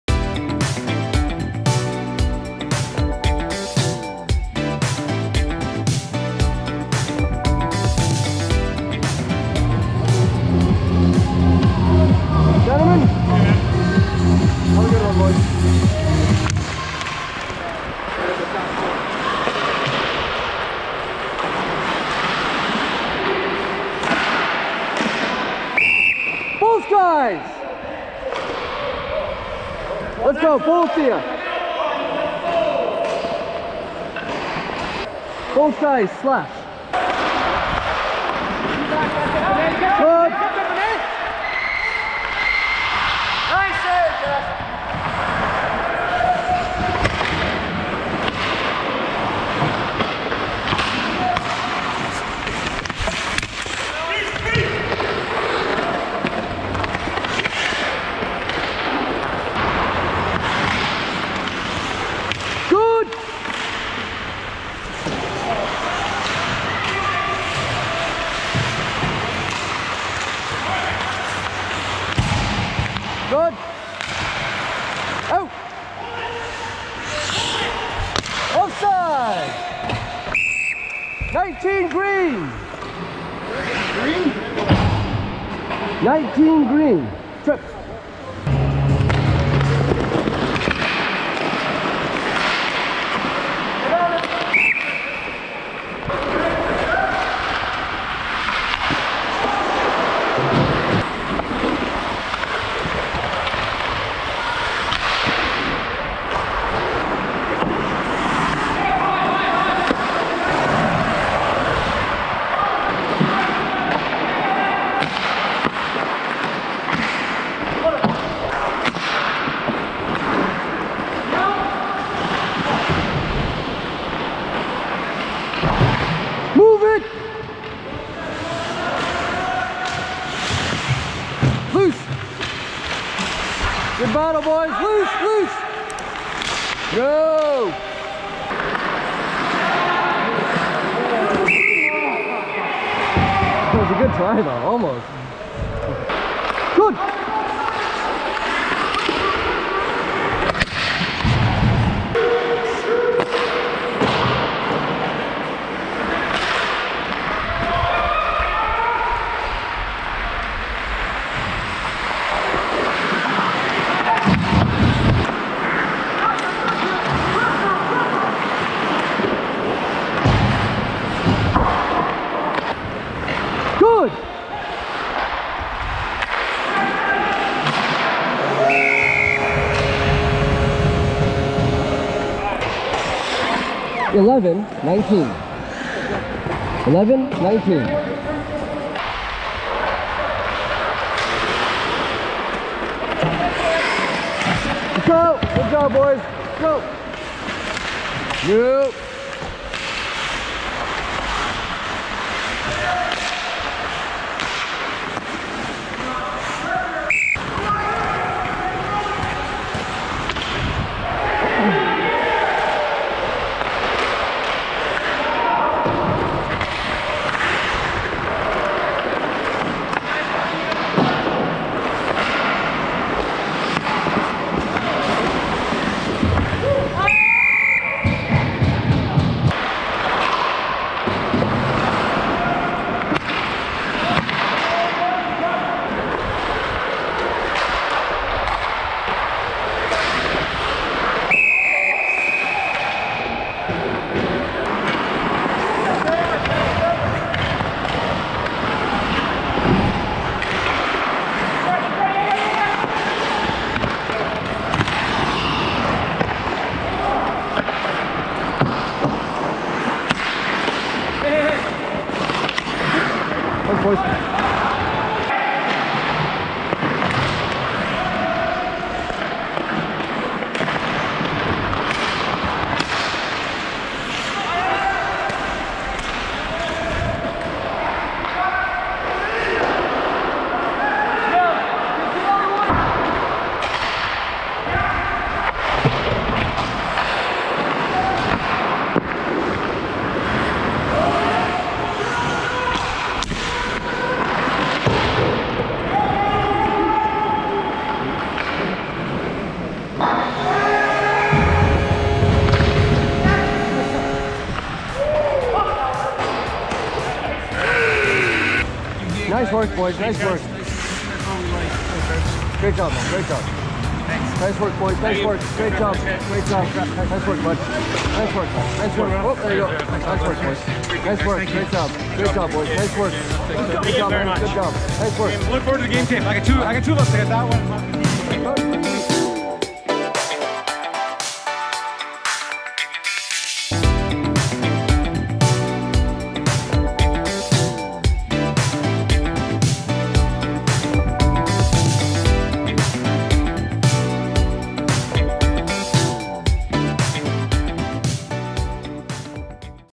Big Clapper - Hey Stripes! The Micd Up GoPro Hockey Refcam - Winter 2019/20 Game 7
Today on Hey Stripes! The Micd Up GoPro Hockey Refcam, we get the windup, the sick flex, the follow through, but do we get the celly?

HS DIV 6